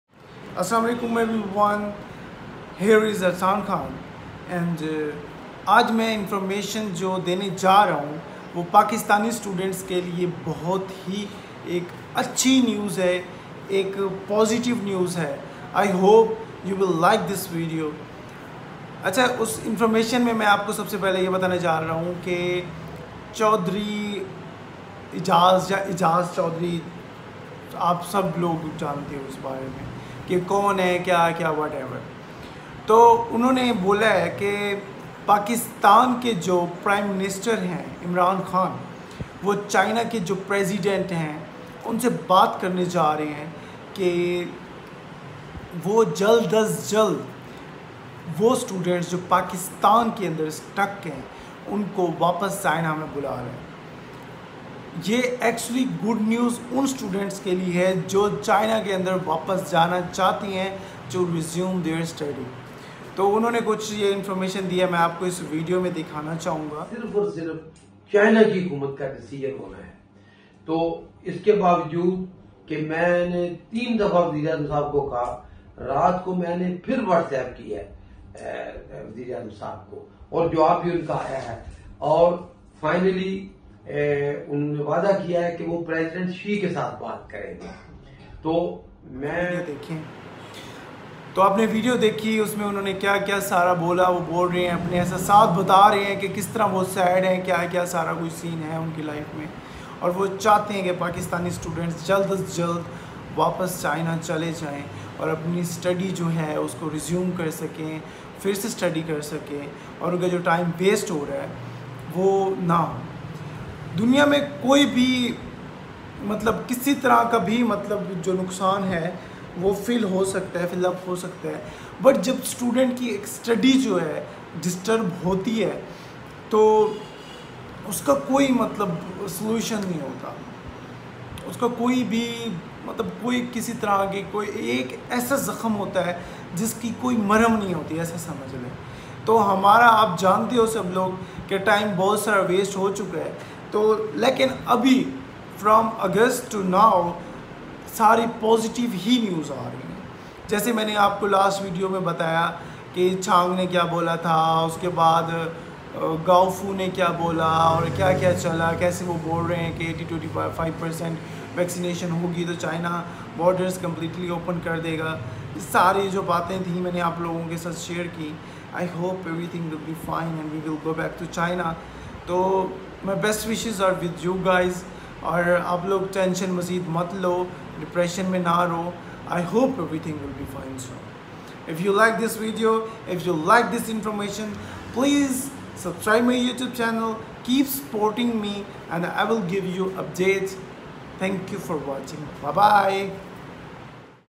Aijaz chaudhary said Imran khan will talk with China soon and Pakistani 🇵🇰 students will go back
Aijaz Chaudhry said imran khan is going to talk with Chinese president and Pakistani students who are stuck in Pakistan from more then 2 years they will able to go back China.its very good news for Pakistani students. may be soon Pakistani students will go back to China